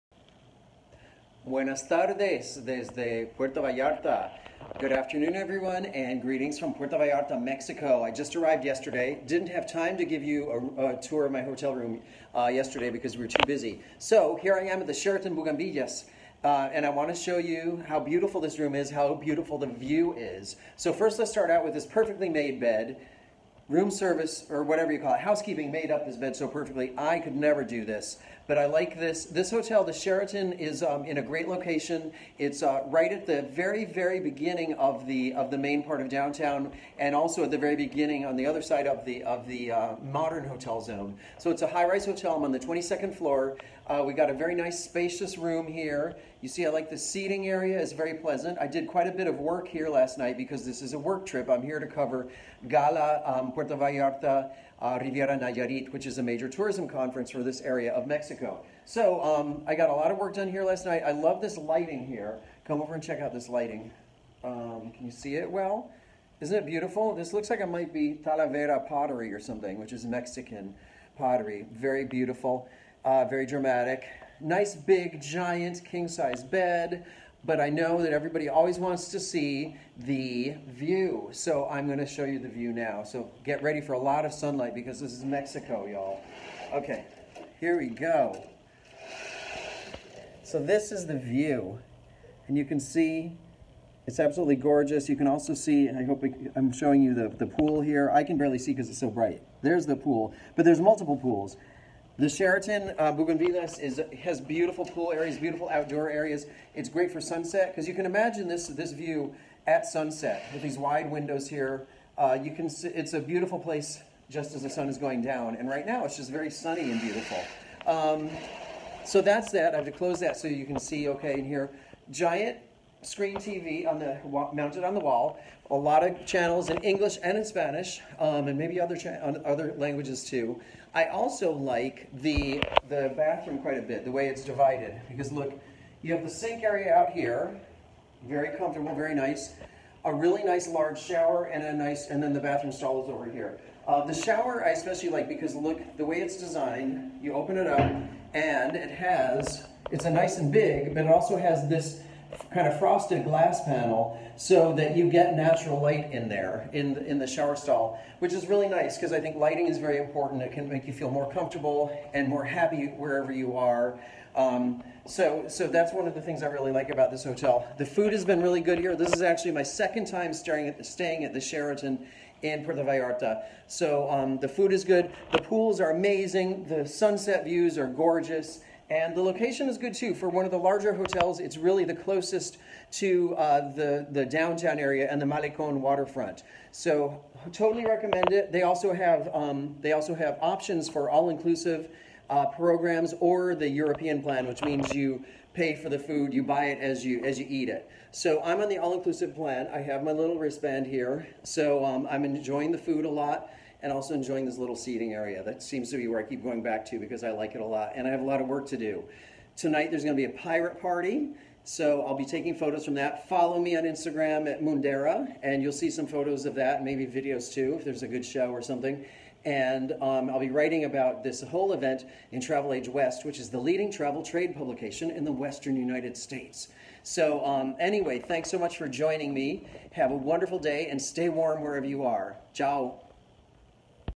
Sheraton Buganvilias Puerto Vallarta — Love My Hotel Room!
In this video hotel room tour and hotel review, I show off the main features and also the beautiful view. One of my favorite things about this hotel is that it's the closest to downtown of all the large-scale Mexico resort hotels in Puerto Vallarta. Find more Mexico travel tips and hotel reviews on my travel blog!